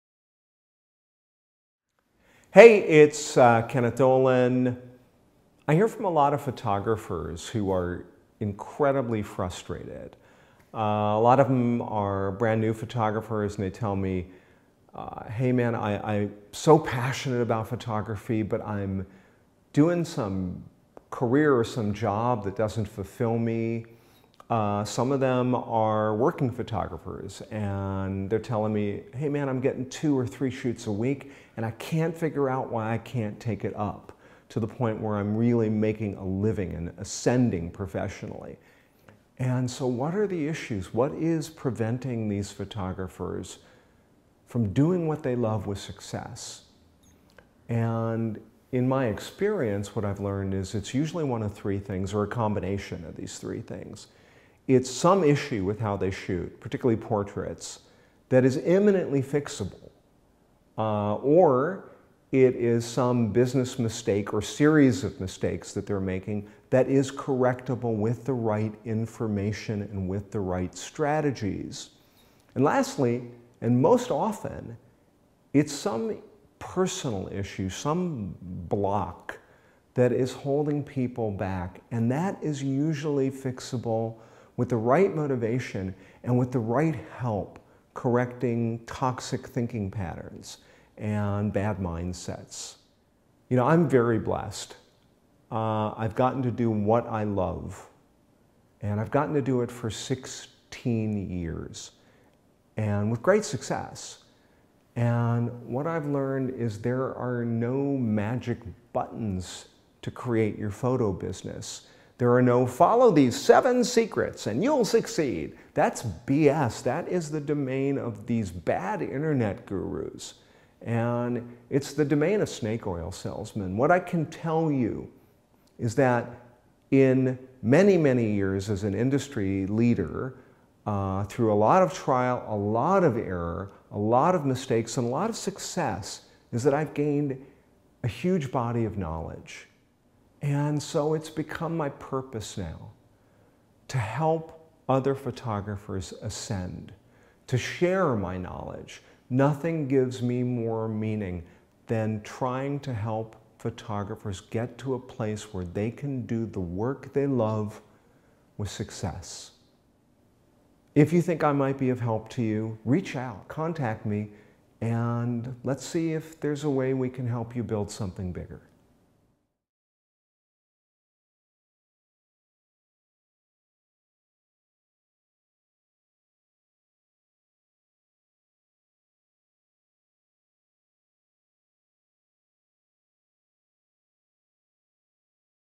For Photographers: What is Holding You Back?
Kenneth discusses how most photographers who come to him are frustrated because they're not getting their photo business working, or monetizing their photography businesses. But this is usually due to simple, imminently fixable problems. And they almost always fall into one of 3 areas. Kenneth discusses his approach, his passion for helping, and why most Instagram-Guru internet "photographer coaches and workshops help themselves, not their clients with too-good-to-be-true simplifications. Here, Kenneth discusses the REAL work behind building a successful photography business.